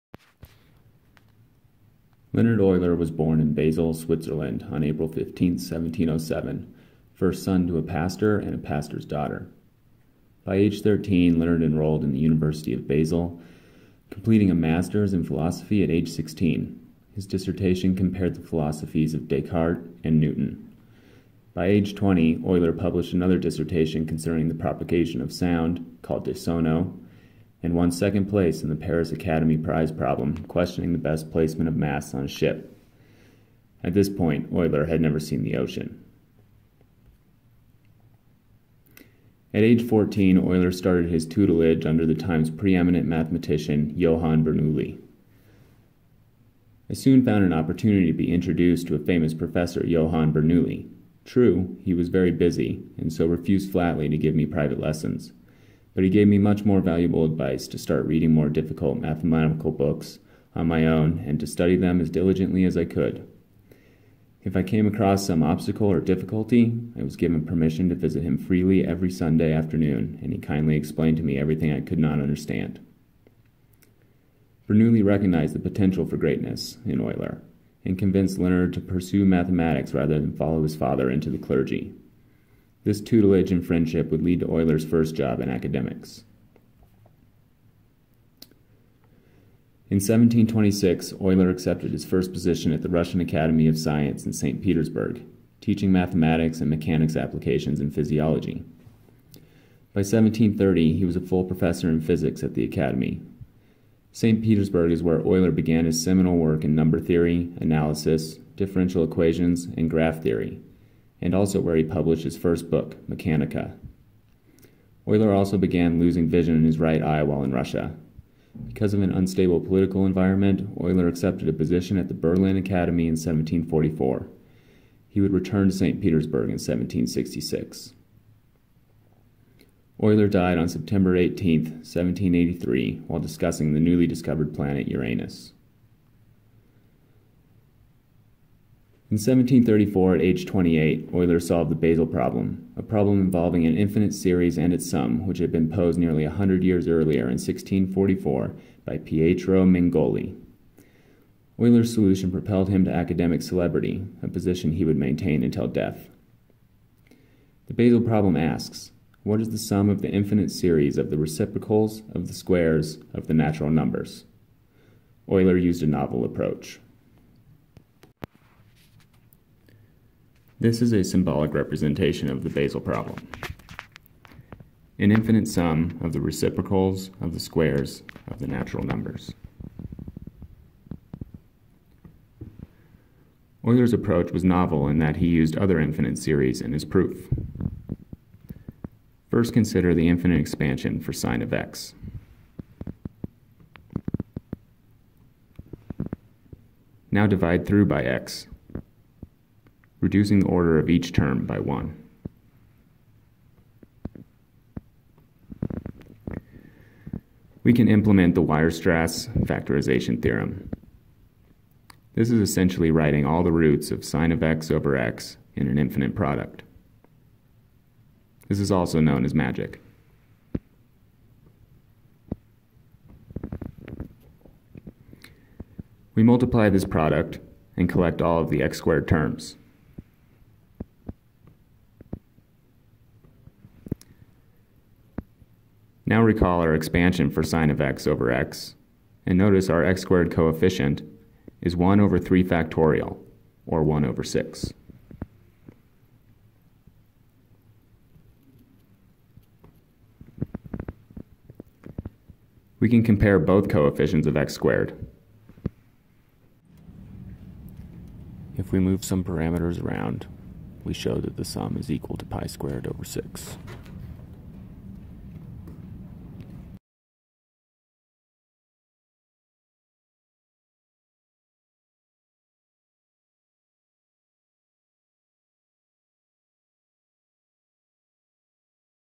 Leonhard Euler and the Basel Problem
Brief history of Leonhard Euler and his solution to the Basel Problem.